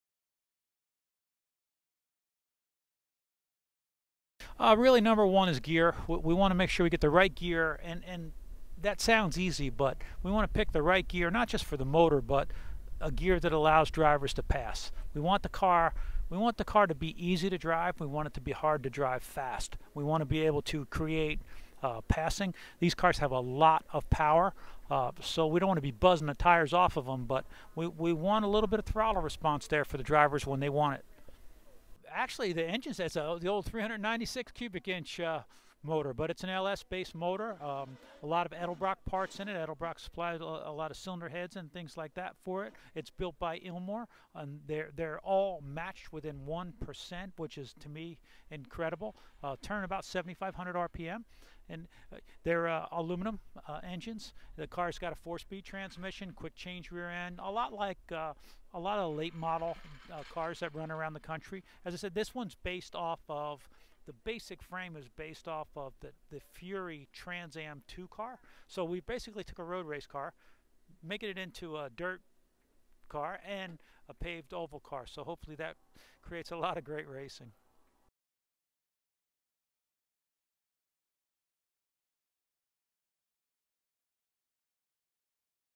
SRX Test at Knoxville Raceway | Ray Evernham
What size engine is in the all new SRX race car? Series co-founder Ray Evernham gives us a brief technical review of the car before a test session on May 10, 2021.